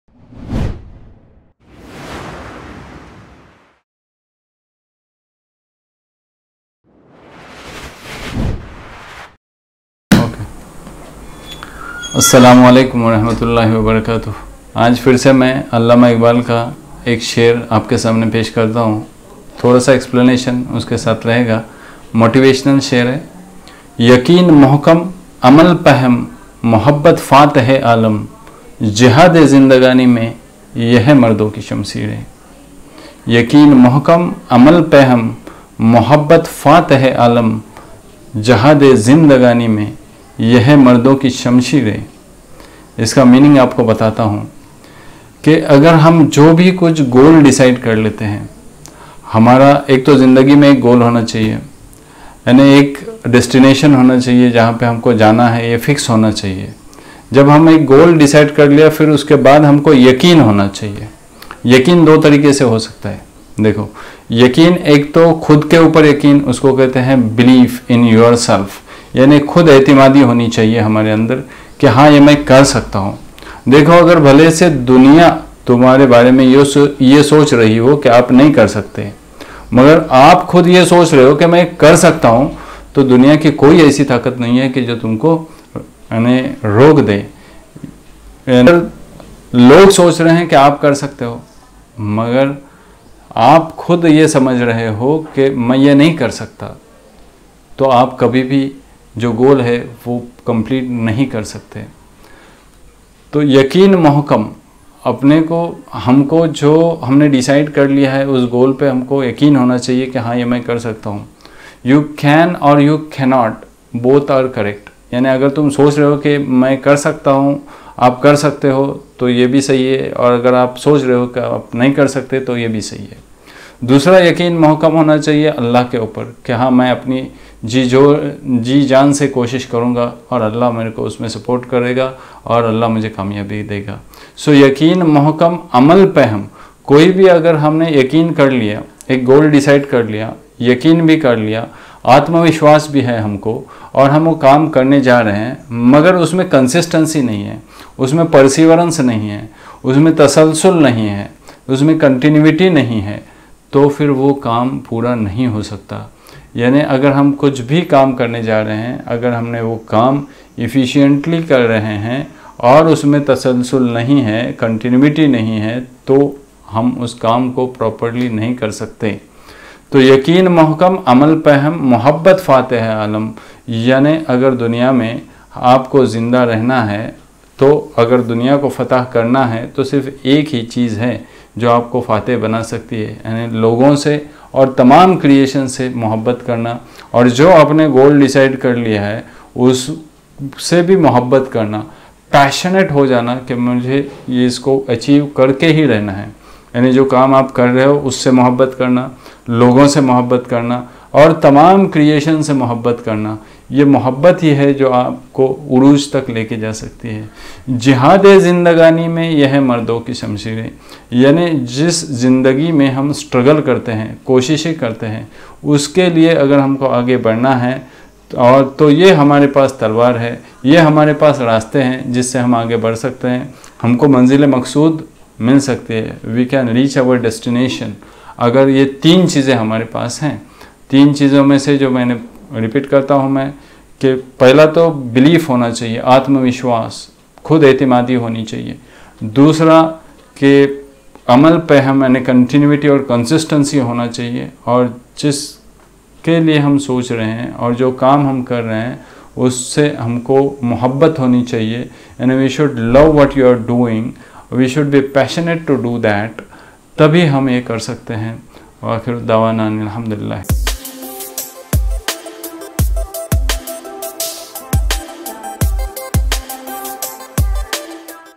ALLAMA IQBAL SHAYARI YAQEEN MOHKAM AMAL PAIHAM" BY PROF.DR.ANTULE A.S.N.
THIS IS SHER BY ALLAMA IQBAL POETRY WHICH BOOST OUR MORALE TO DO WHAT WE HAVE DECIDED TO DO.
" YAQEEN MOHKAM , AMAL PAIHAM, MOHABBAT FAATAHEY AALAM
JIHAADEY ZINDAGAANI MEY YE HAIN MARDO KI SHAMSHEEREIN.